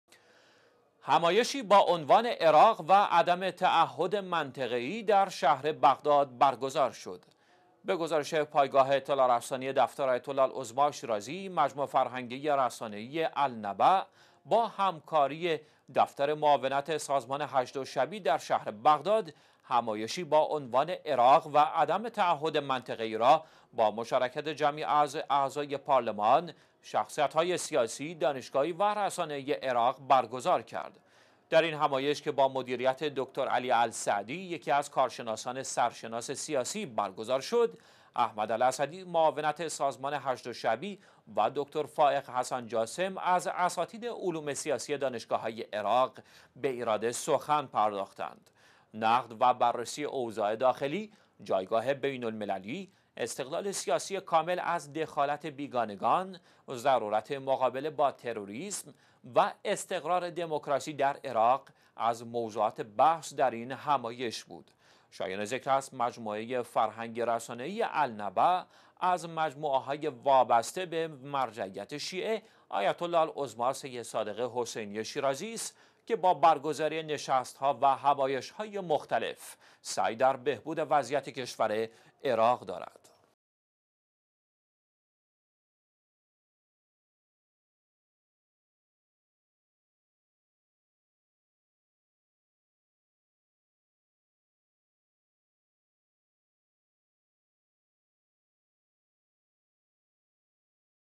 همایش بررسی شرایط داخلی و بین المللی عراق، در بغداد / ۳۰ اسفند ۱۳۹۵
همایش بررسی شرایط داخلی و بین المللی عراق، در بغداد

همایشی با عنوان «عراق و عدم تعهد منطقه ای» در شهر بغداد، برگزار شد.

به گزارش پايگاه اطلاع رساني دفتر آیت_الله_العظمی_شیرازی، مجموعه فرهنگی ـ رسانه ای «النبأ» با همکاری دفتر معاونت سازمان حشدالشعبی در شهر «بغداد» همایشی تحت عنوان «عراق و عدم تعهد منطقه ای» با مشارکت جمعی از اعضای پارلمان این کشور، شخصیت های سیاسی، دانشگاهی و رسانه ای عراق، برگزار کرد.

در این همایش که با مدیریت «دکتر علی السعدی» ـ یکی از کارشناسان سرشناس سیاسی ـ برگزار شد؛ «احمد الاسدی» معاونت سازمان حشد الشعبی و «دکتر فائق حسن جاسم» از اساتید علوم سیاسی دانشگاه های عراق، به ایراد سخن پرداختند.

نقد و بررسی اوضاع داخلی عراق، جایگاه بین المللی، استقلال سیاسی کامل از دخالت بیگانگان، ضرورت مقابله با تروریسم و استقرار دموکراسی در کشور، از موضوعات بحث در این همایش بود.

شایاین ذکر است، مجموعه فرهنگی ـ رسانه ای «النبأ» از مجموعه های وابسته به مرجعیت شیعه «آیت الله العظمی سید صادق شیرازی»مدظله، است که با برگزاری نشست ها و همایش های مختلف، سعی در بهبود وضعیت کشور عراق دارد.

الإثنين - ٢١ جمادي الثانيه ١٤٣٨